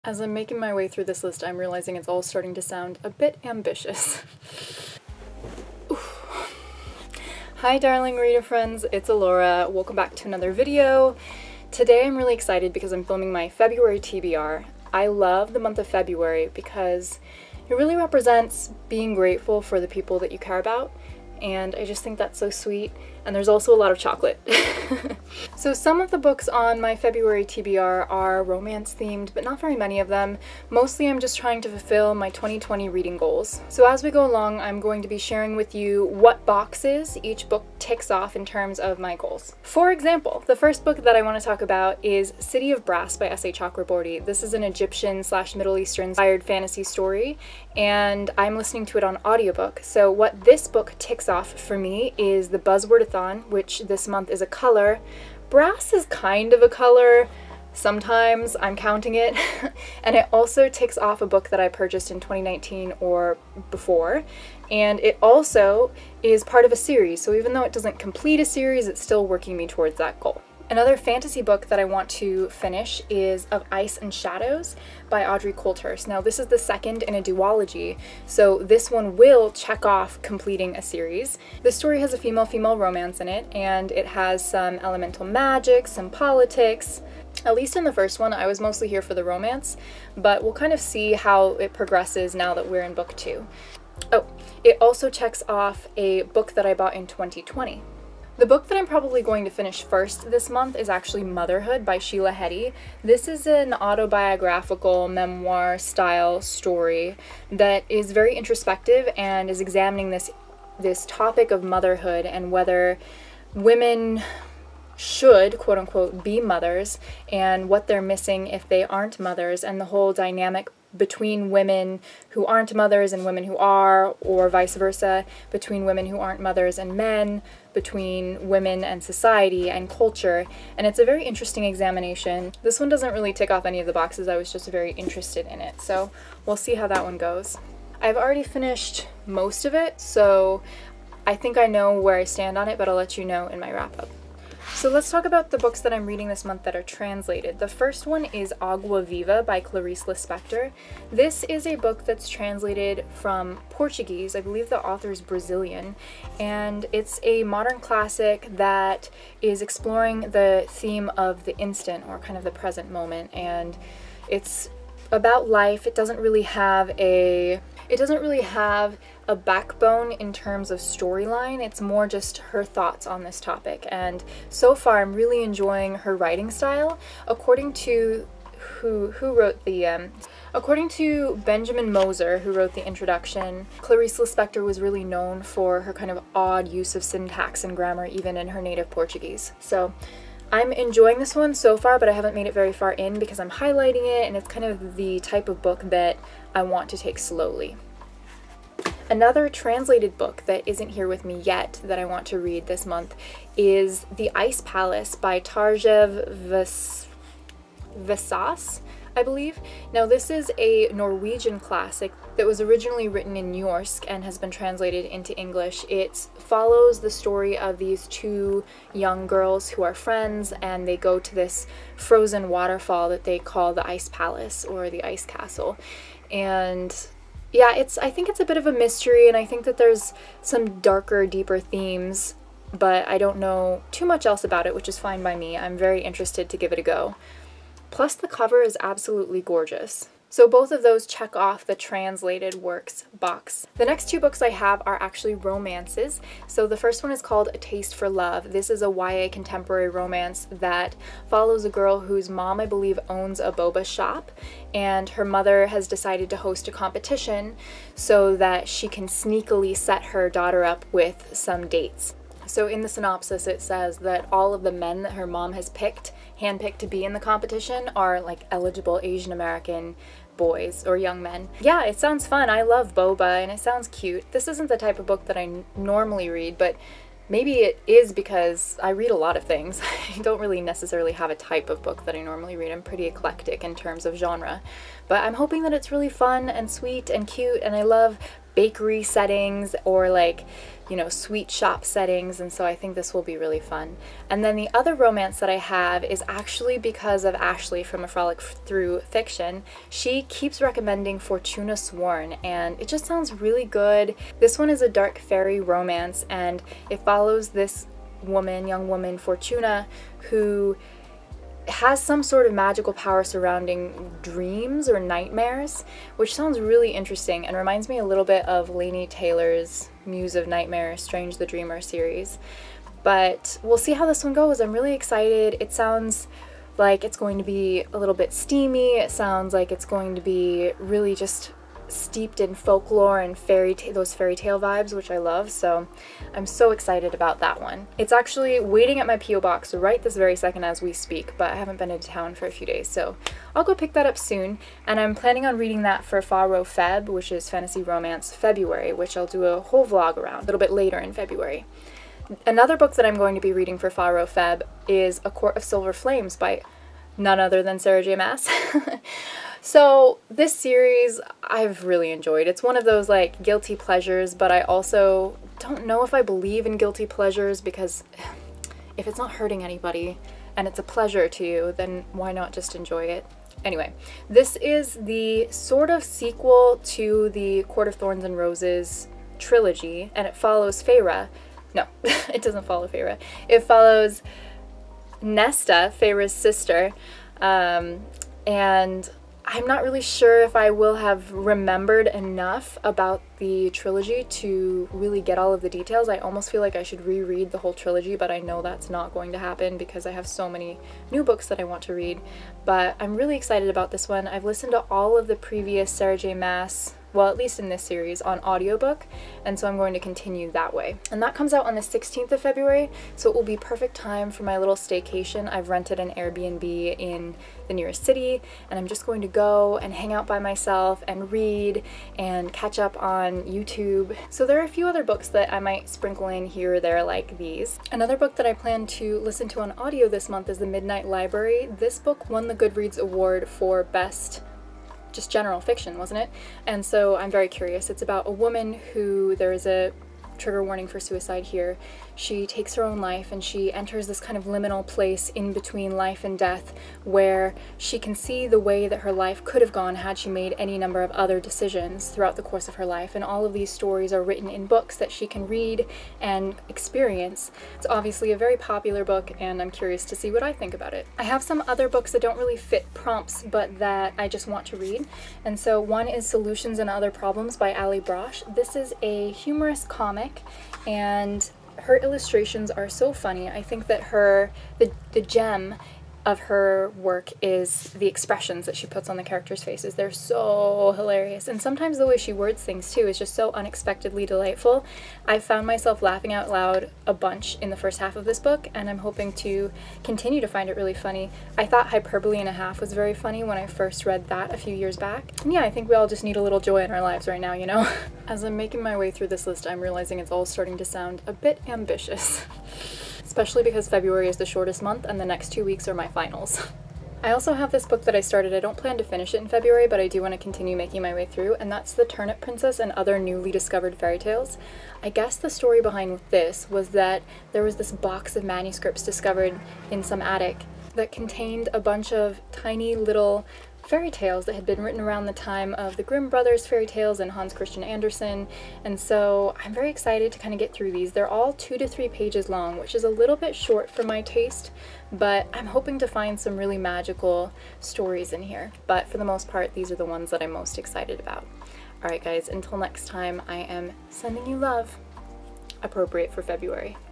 💜February TBR💜
Or make friends with me on Goodreads: @AloraSmilez

0:00 intro
0:40 city of brass
1:10 of ice and shadows
1:38 motherhood
2:24 agua viva
3:18 the ice palace
4:03 a taste for love
4:54 fortuna sworn
5:51 a court of silver flames
7:07 the midnight library
7:43 solutions & other problems
8:27 the turnip princess